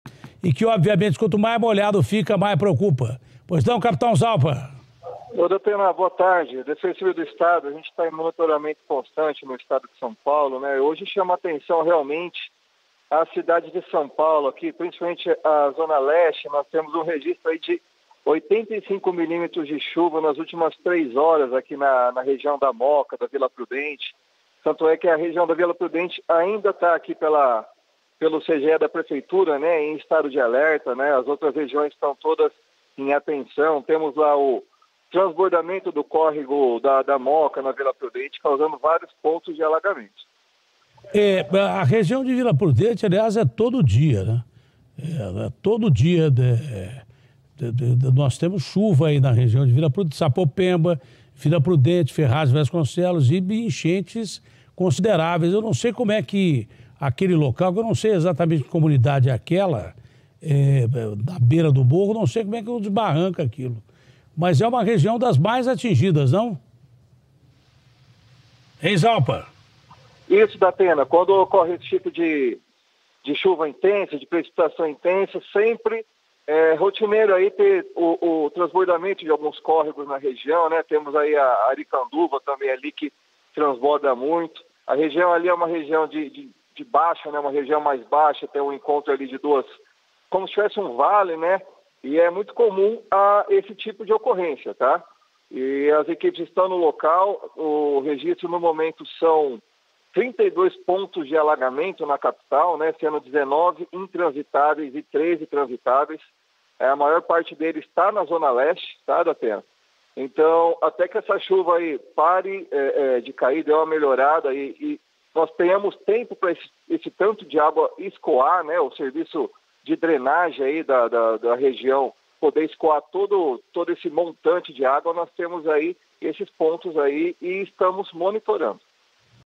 Defesa Civil do Estado explica situação das chuvas intensas em SP
Capitão Zaupa, da Defesa Civil do Estado, explica situação das chuvas intensas que atingiram a cidade de São Paulo na tarde desta segunda-feira (13).